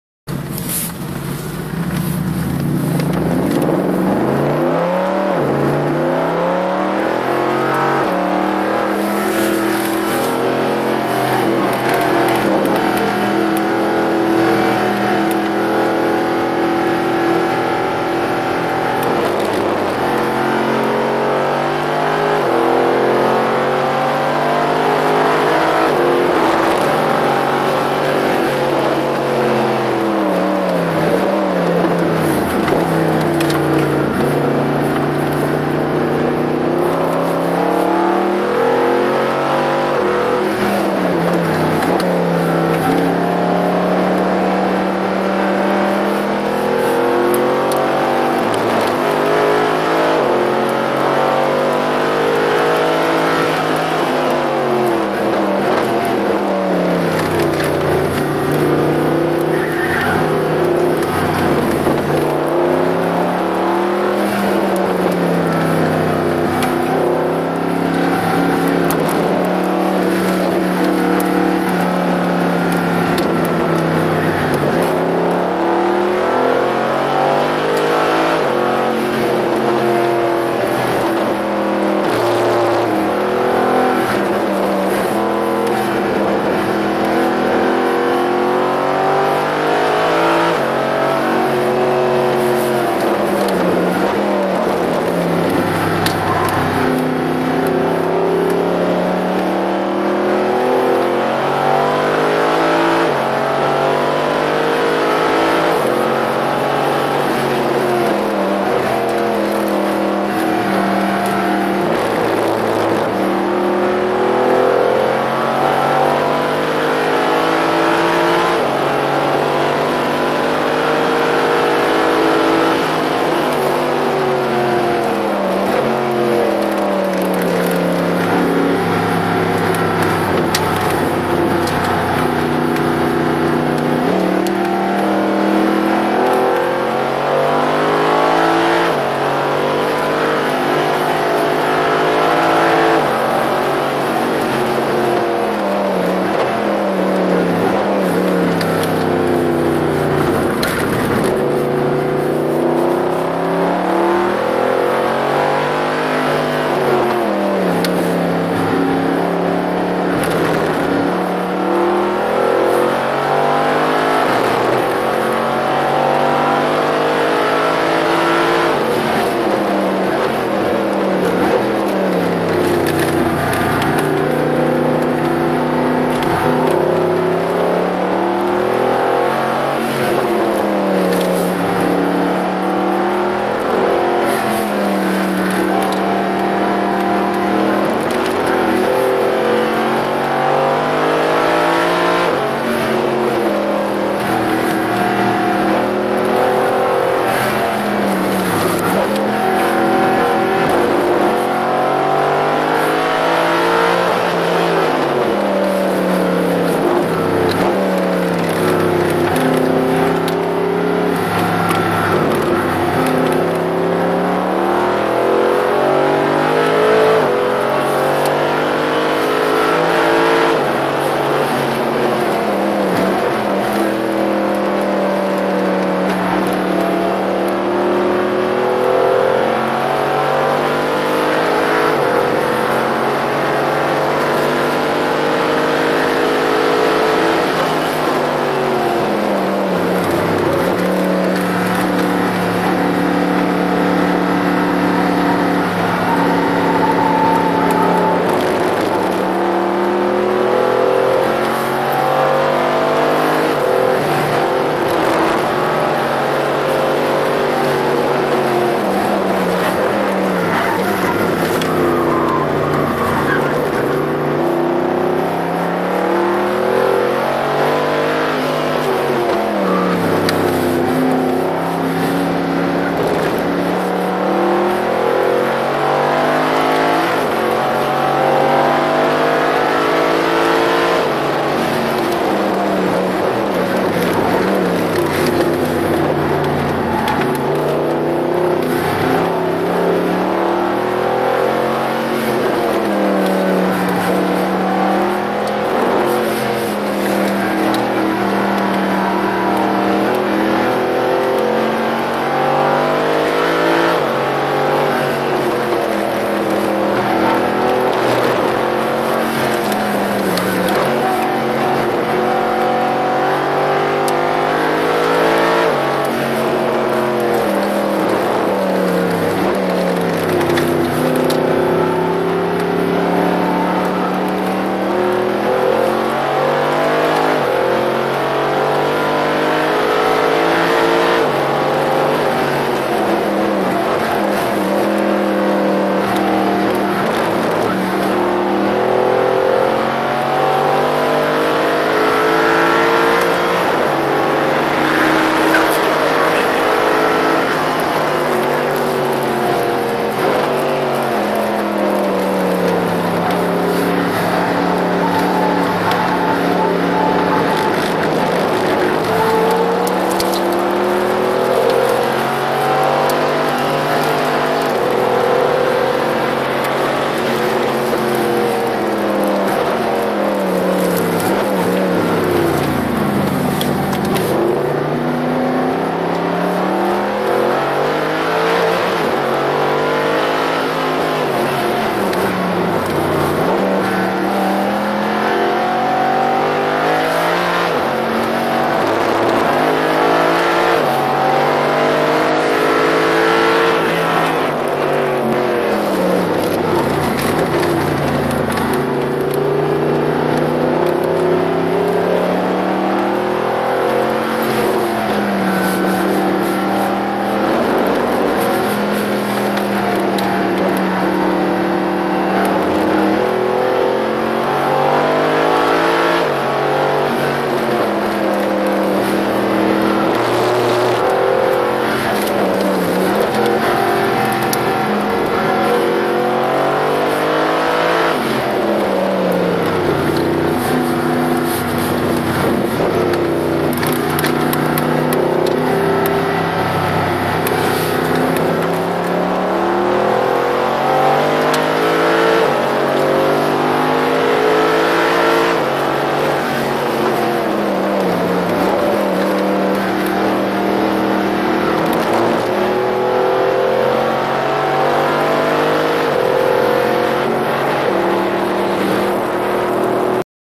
Tor Poznań Track Day 9.03.2014 - Mercedes SLS Onboard!
620KM ale 900konny GT-R wziął nas jak małe ciotki :D